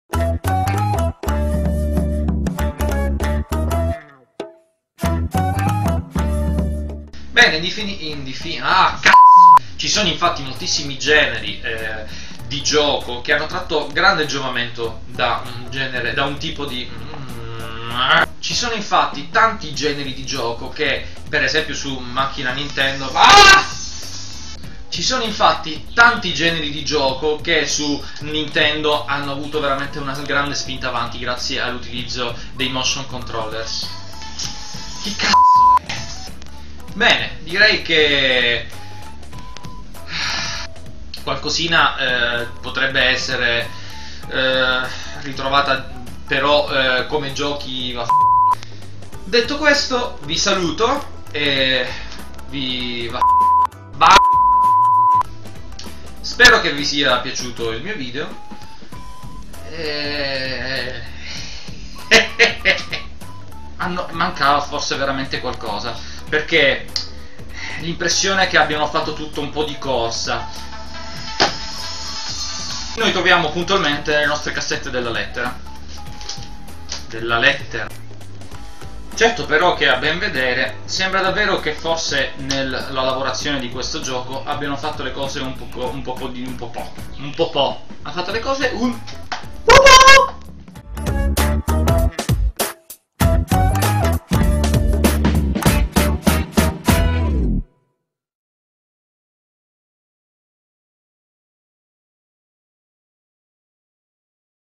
BACK IN TIME 2010 - OUT TAKES - Playstation MOVE - ITA
Torniamo indietro nel tempo ai primi video del sottoscritto.
Questa volta vi presento i fuori onda dei video dedicati ai primi tempi di Playstation MOVE e ai giochi relativi.

I filmati a cui si riferiscono questi out takes sono principalmente la recensione di Playstation MOVE

Buona visione! E che il rage sia con voi!